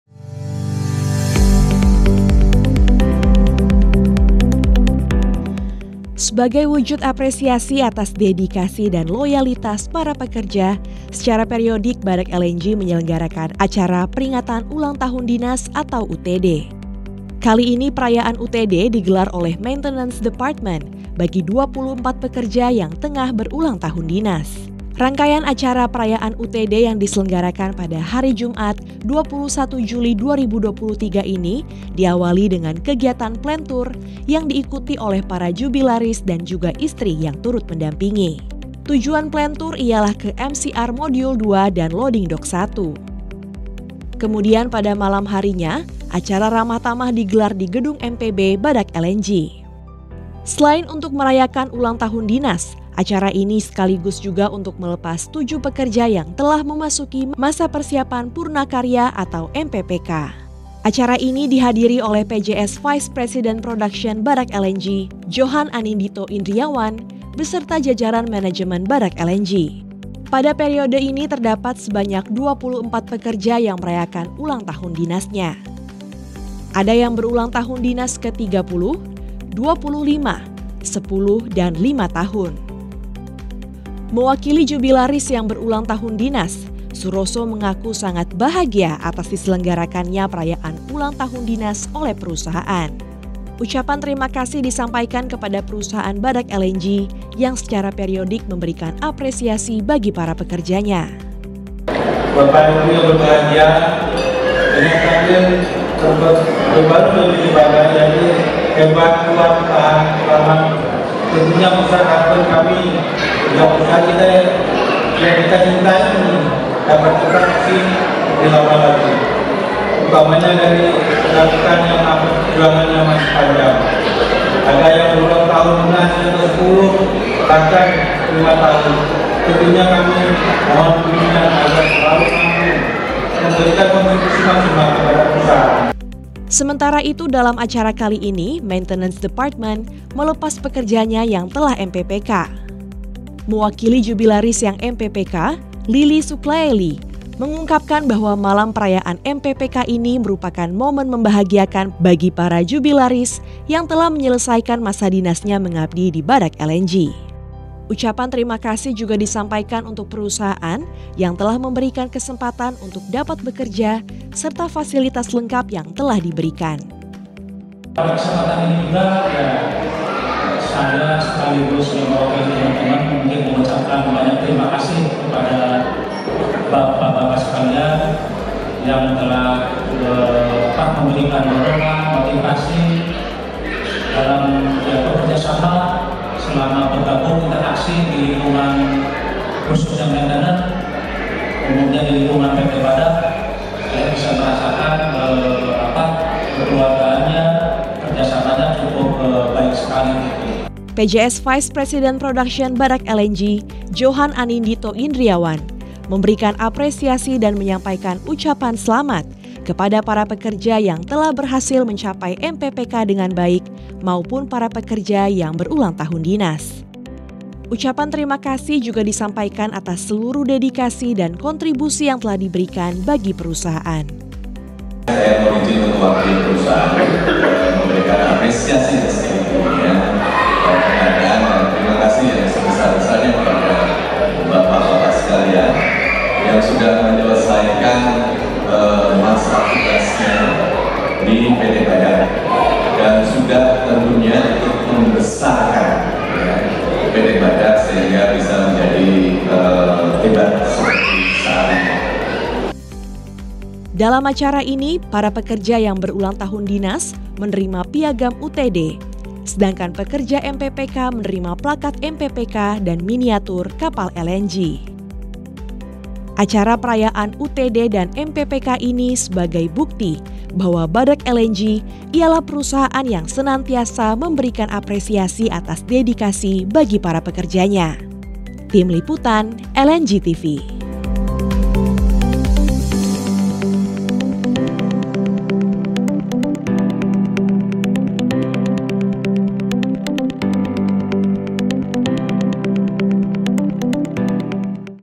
Maintenance Dept. Rayakan UTD & MPPK Pekerja
Maintenance Department telah menyelenggarakan acara Perayaan Ulang Tahun Dinas Pekerja dan Pelepasan Pekerja yang telah memasuki masa Persiapan Purna Karya pada Jumat (21/07/23) lalu. Acara Perayaan ini sebagai bukti bahwa Badak LNG ialah Perusahaan yang senantiasa memberikan Apresiasi atas Dedikasi Para Pekerjanya.